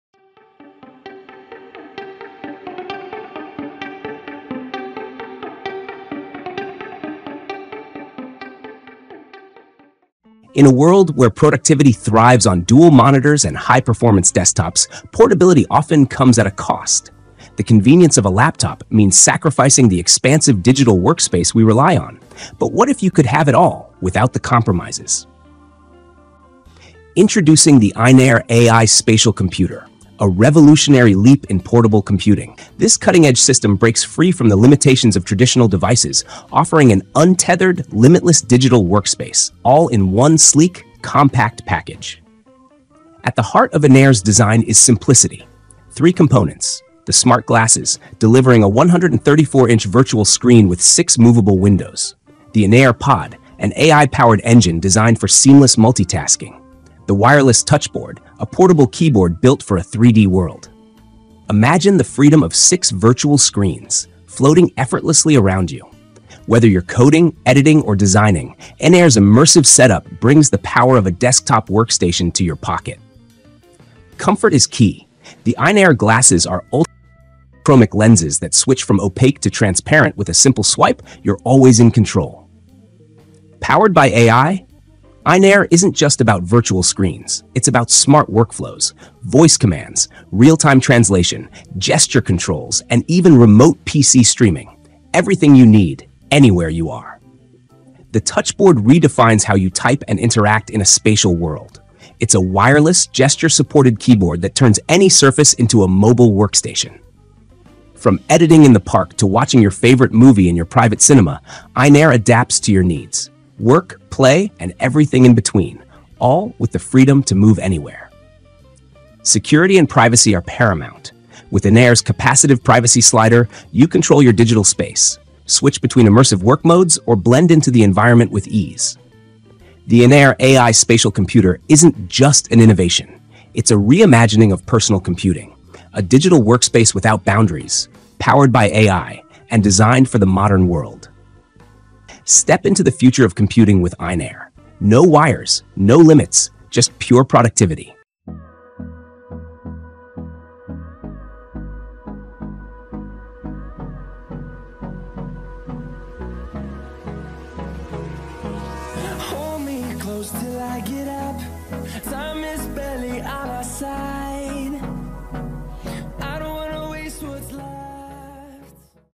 This AR Glasses Set Turns ANY Surface into a 134" Virtual Workspace! Meet INAIR AI Spatial Computer
Discover the future of portable computing with the INAIR AI Spatial Computer! This groundbreaking system combines smart glasses, an AI-powered computing hub, and a gesture-responsive keyboard to create a limitless digital workspace. Experience up to six floating virtual screens, seamless PC streaming, and intuitive 3D navigation—all from a sleek, lightweight design. Whether you're coding, designing, or enjoying entertainment, INAIR adapts to your needs. Say goodbye to traditional monitors and hello to a borderless digital experience.